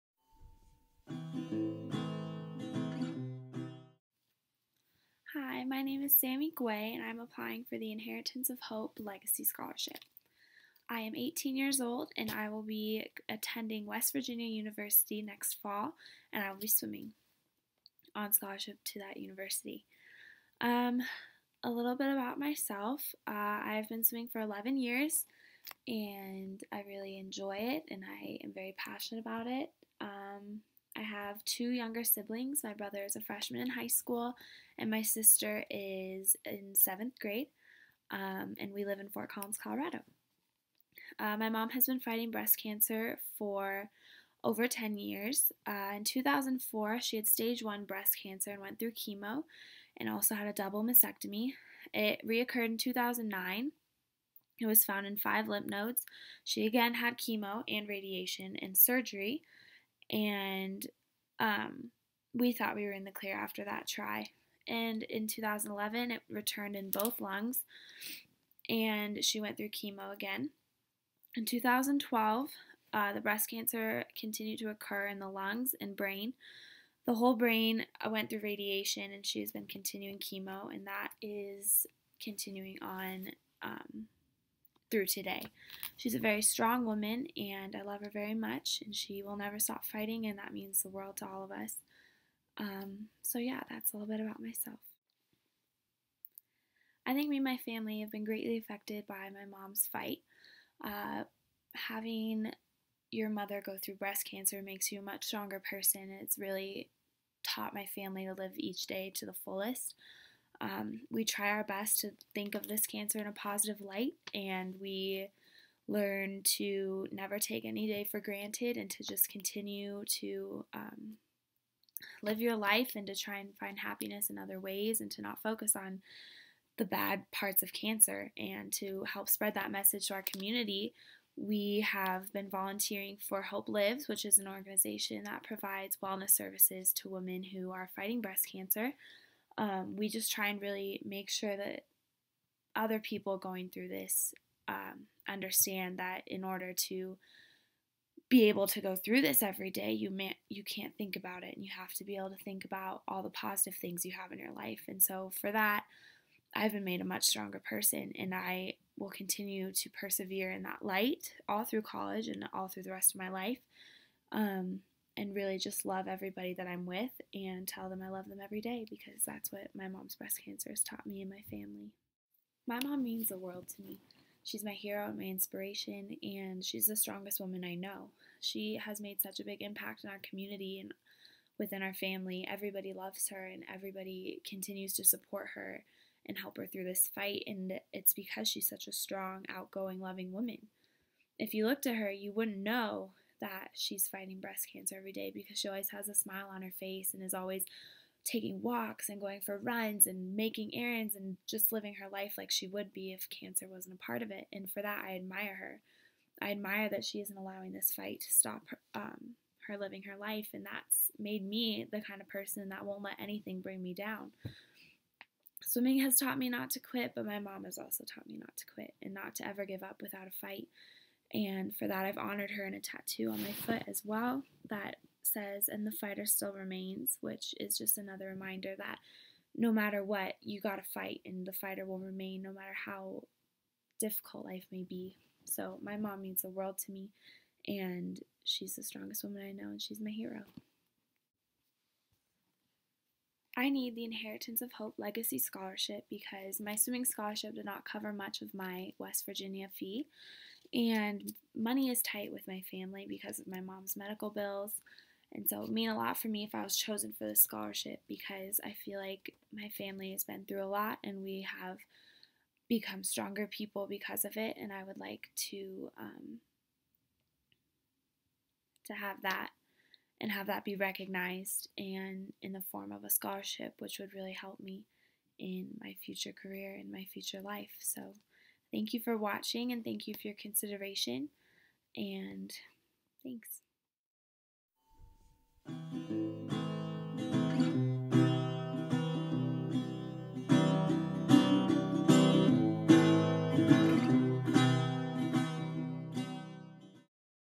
2014 Legacy Scholarship Winner Sammie Guay
Inheritance of Hope's Legacy Scholarship is awarded to college-bound high school seniors living with a parent having a life-threatening diagnosis. Interested donors can name a Legacy Scholarship in honor or memory of a loved one